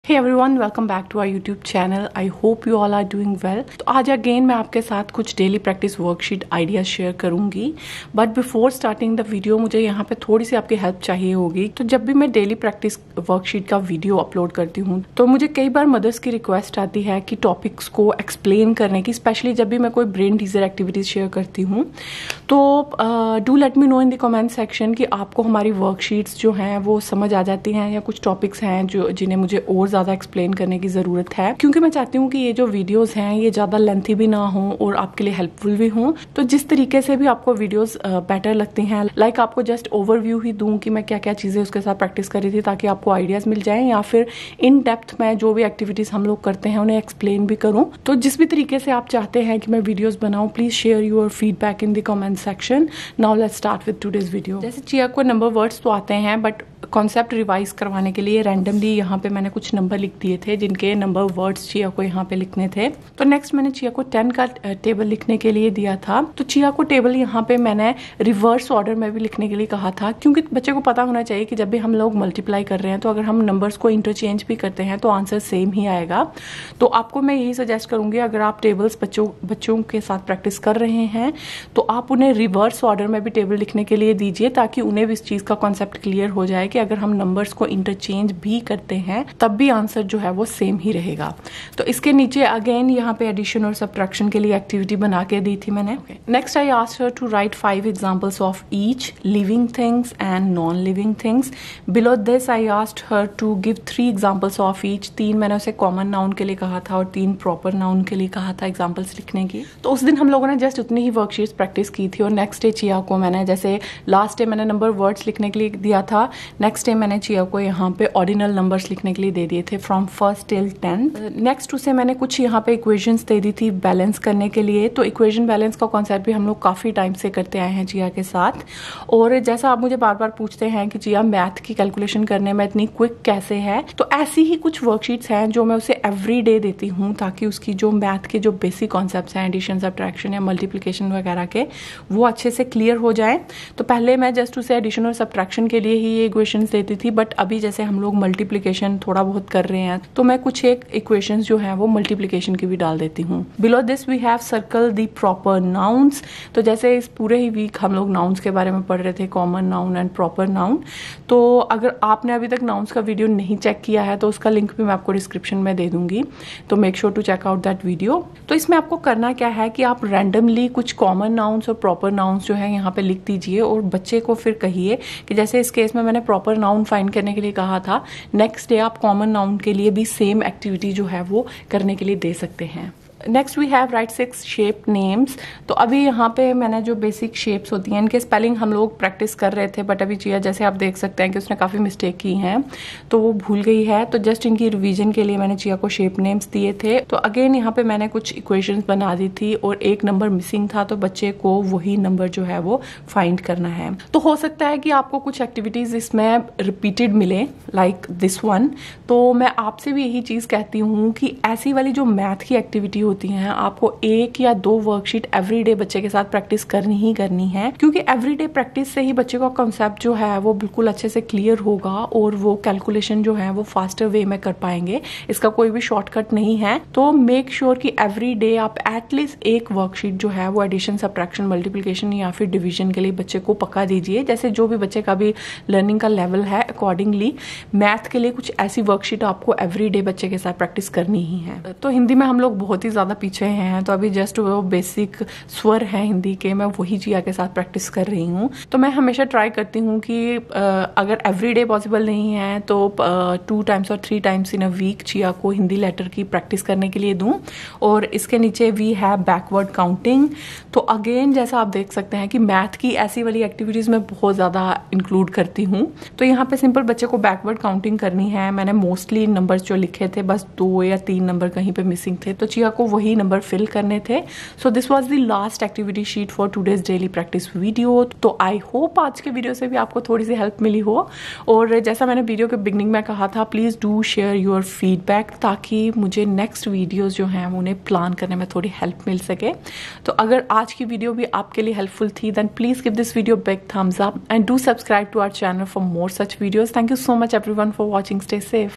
Homeschool/After School Written Practice Worksheet Ideas for 6-7 year Kids (Day 6-7)
All these activities can be done easily by 6-7 year kids however, you can make these sheets easy or difficult according to your child's learning pace or age group.